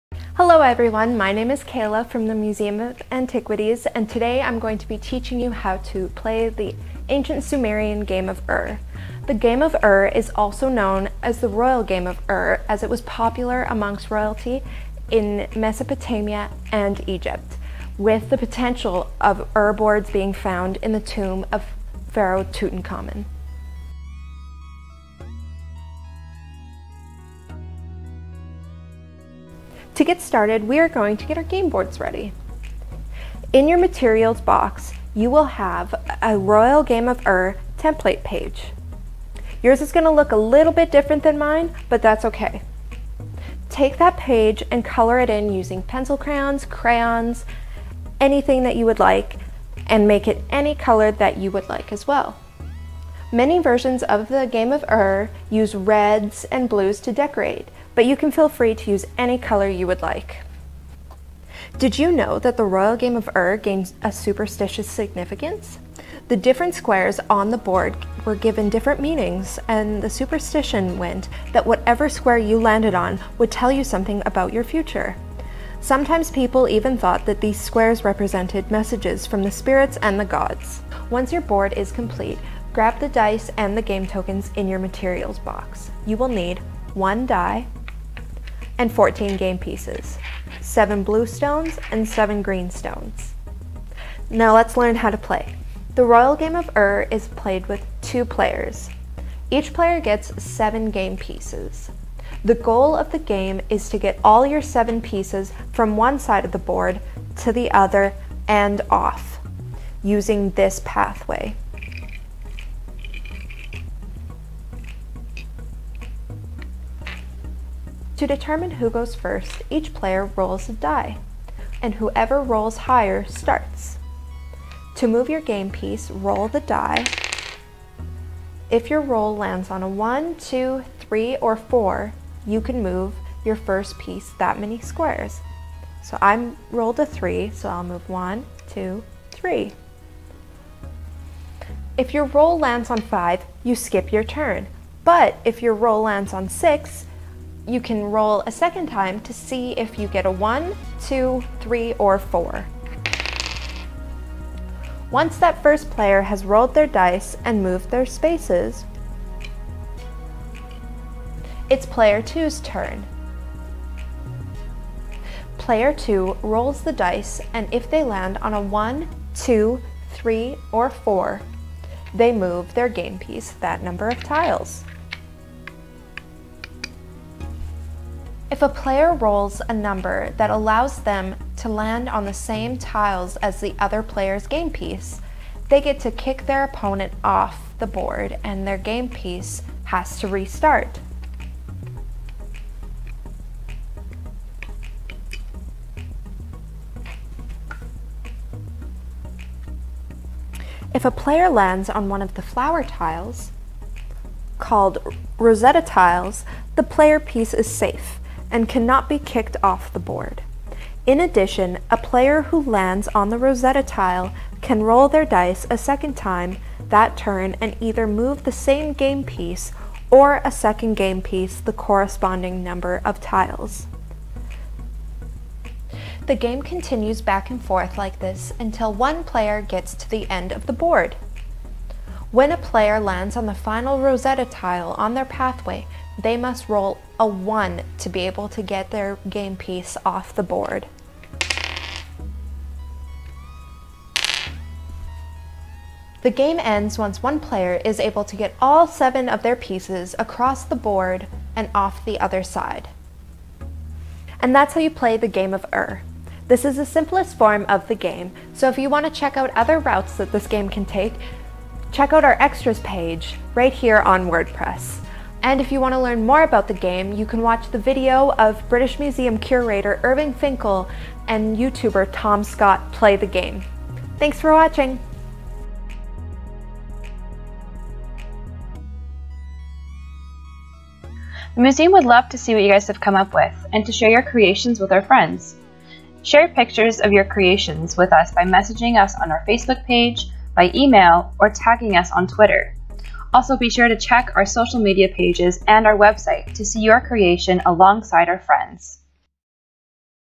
The Royal Game of Ur Tutorial
Learn how the play the game of Ur that originated from ancient Mesopotamia in this camp video.